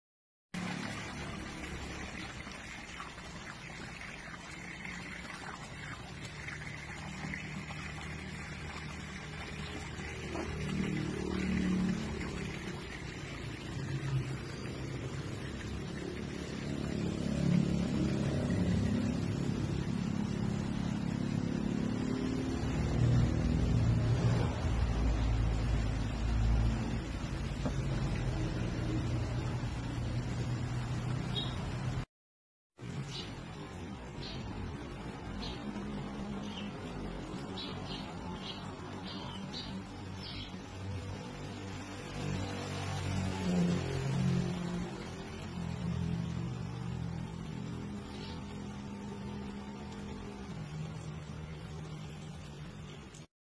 amazing fish swimming
ang amazing fish ay palaging nag swimming. yon talaga work nila swimming always kasi amazing fish sila.